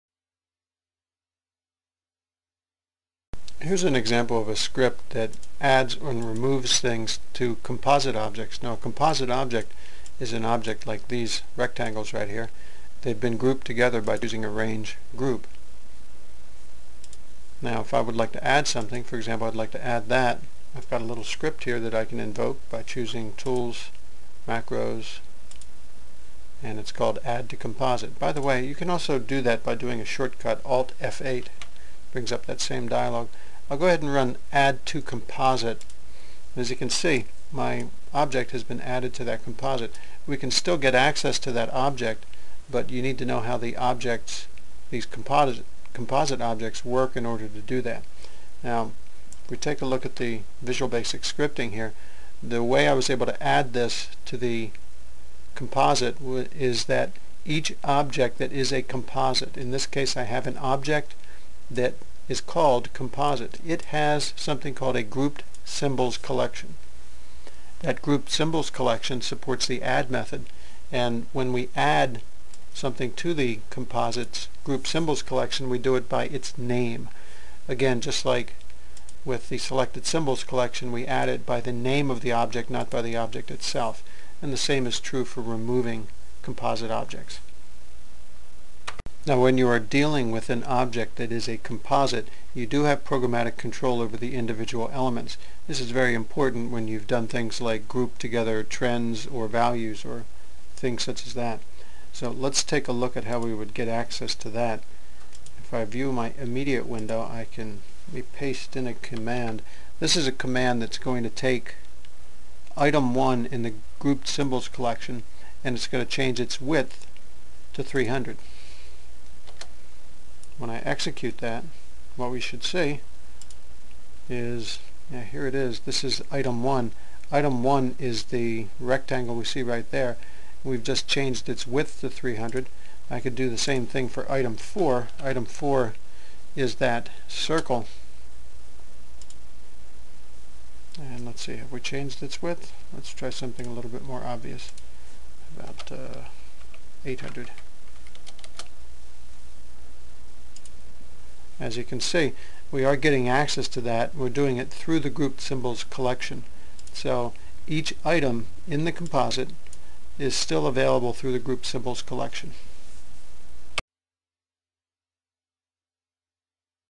OSIsoft: Composite Objects in PI ProcessBook . v1.3
How to work with composite (i.e. grouped) objects in ProcessBook.

you can see where this video is in the sequence of videos on this topic.